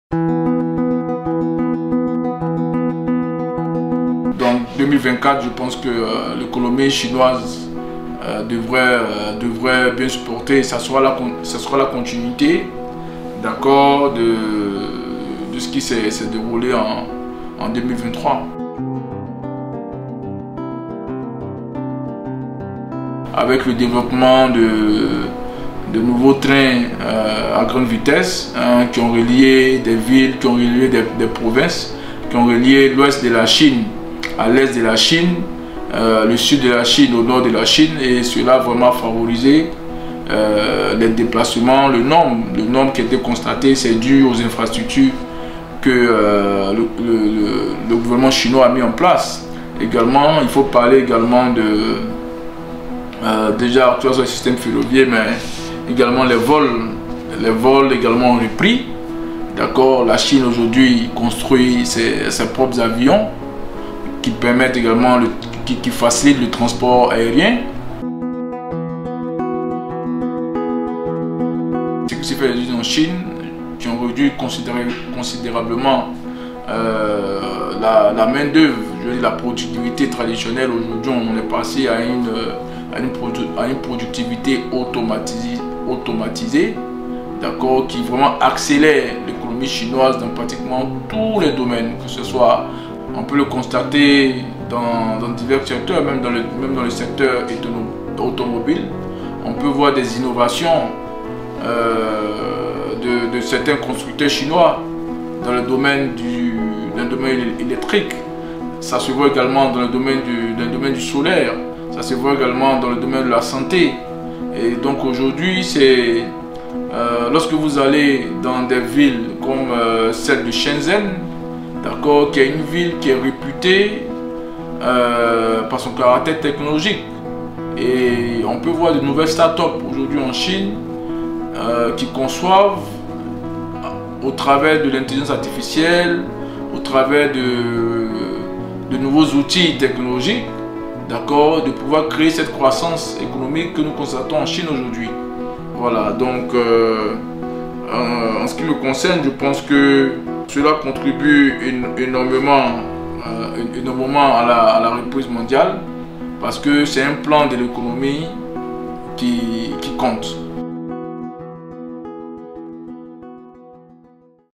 GLOBALink | Expert in Cote d'Ivoire expresses confidence in China's economic growth
An expert in Cote d'Ivoire has shared his outlook on China's economic growth.